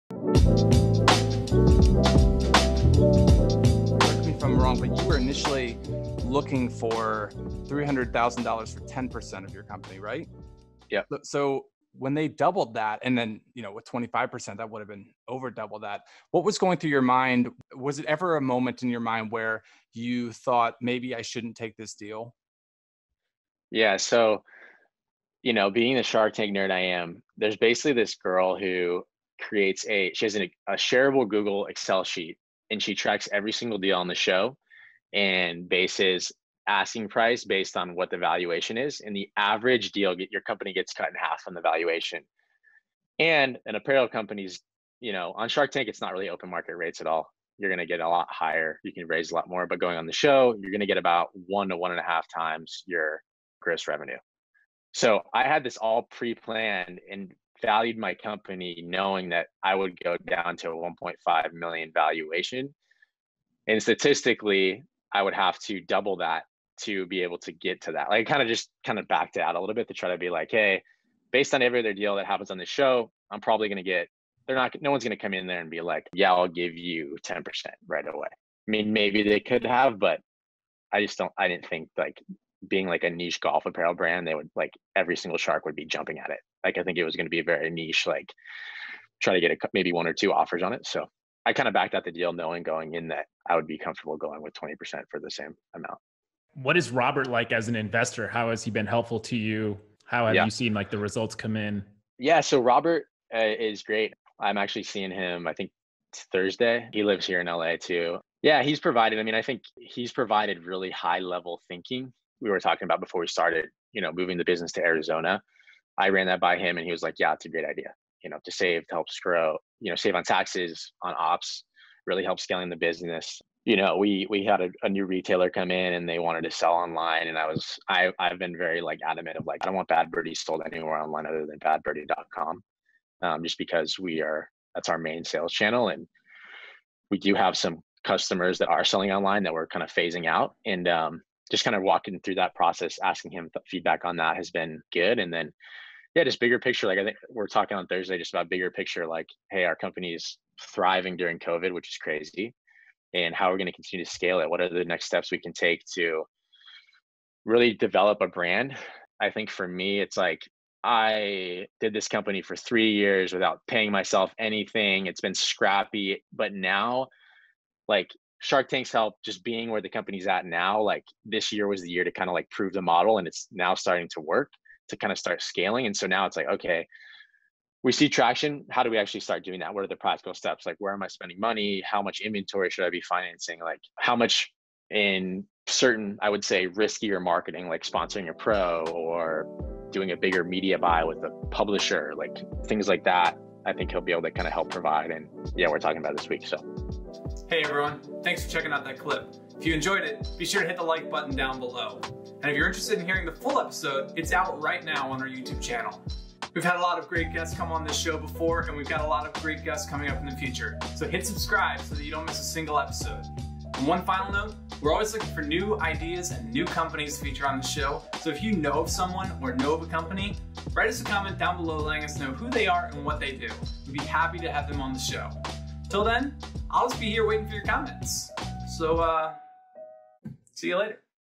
How to Correctly Value Your Company For Shark Tank According to Former Guest, Bad Birdie
Going on Shark Tank is a daunting process.  Even after countless hours of practice, there is still no telling what will happen in the Shark Tank.  Deals are made and equity is given up with investors that have invested in over 500 companies.  It can be a ruthless experience in the depths of the arena.

However, Jason Richardson, founder of Bad Birdie, has figured it out.  Bad Birdie appeared on season 11, episode 18 and was able to lock down a deal with Robert Herjavec for $300,000 for a 20% stake in his company.

He recently came across an analysis that exposed the series for what it is.  A strategy you can use to hack the negotiation process to ensure you walk away with the valuation you wanted from the start.  In this clip of our podcast the founder of Bad Birdie shares this valuable strategy anyone can use to get the most out of Shark Tank.